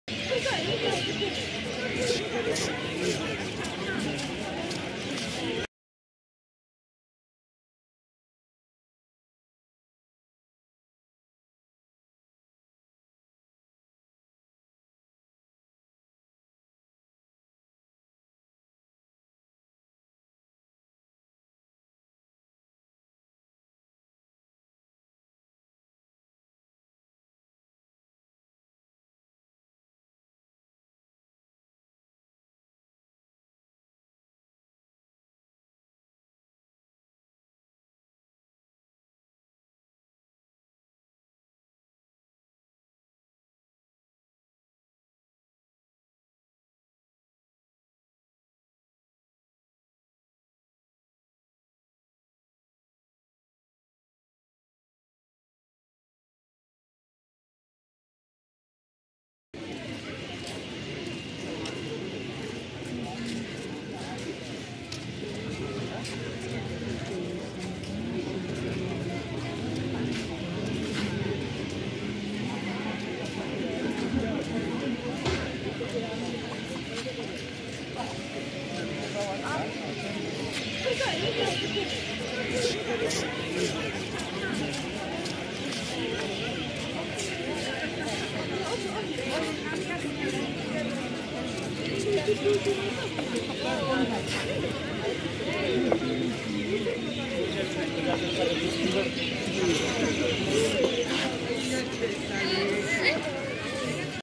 Some monks are meditating outside as the temple is closed now#bodhgaya 2022
2022 bodhgaya Some monks are meditating outside as the temple is closed now